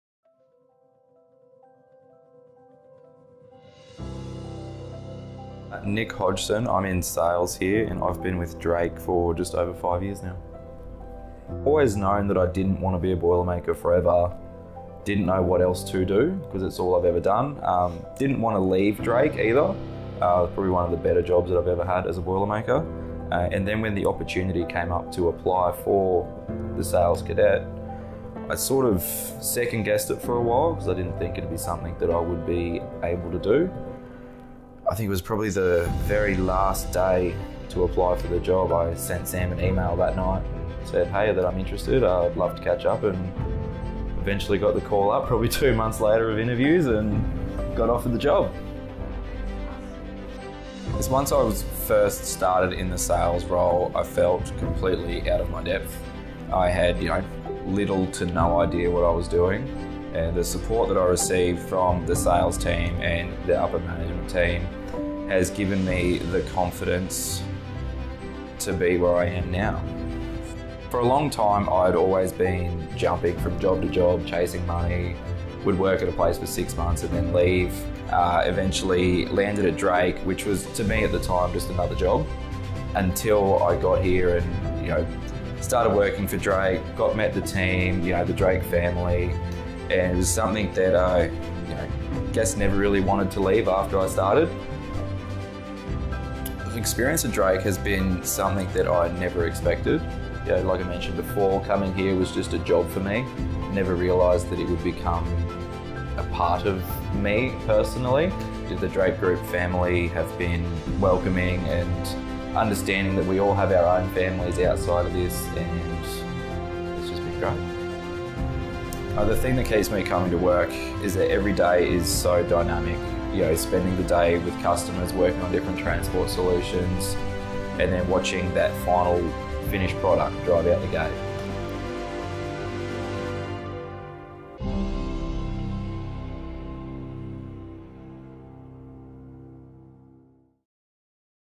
Employee Spotlight: Nick Hodgson | The Drake Group
What’s Nick’s journey with Drake Group been like?

Nick started out as a boilermaker but decided he wanted to make a switch to our sales team, where he’s made a huge difference chasing the best opportunities and keeping the company in the best state possible.

Learn more about Nick and his passion for working with Drake Group.